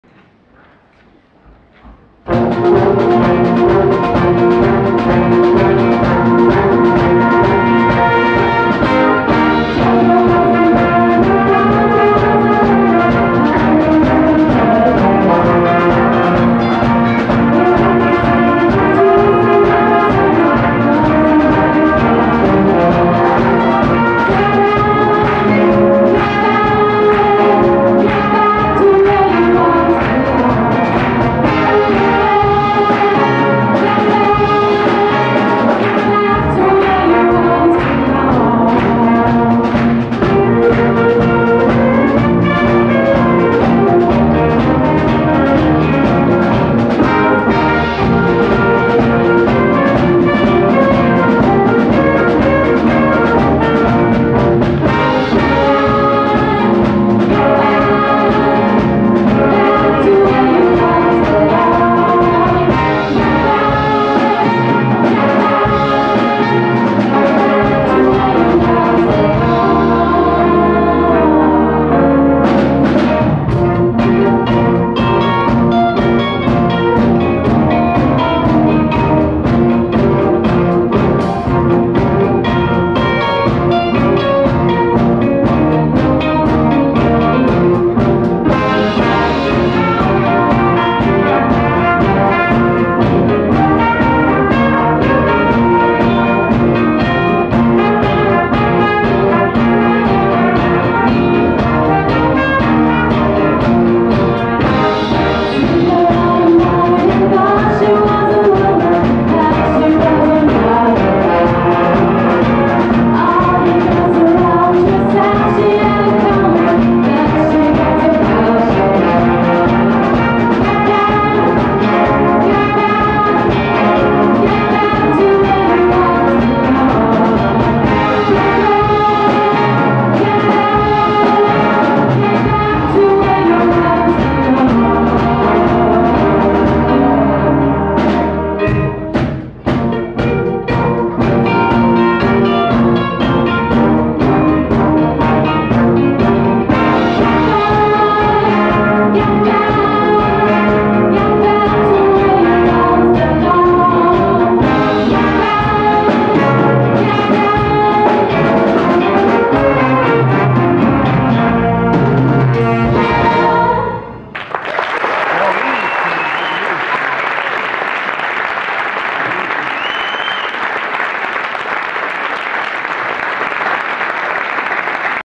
United School Bands 05/26 - Deutsche Schule Rom
Soundlabor Weiz + Big Band Linz (Österreich), Fedde S. Big Band - Heerenveen (Holland), Big Band - Boston (England), Big Band - Rom (Italien)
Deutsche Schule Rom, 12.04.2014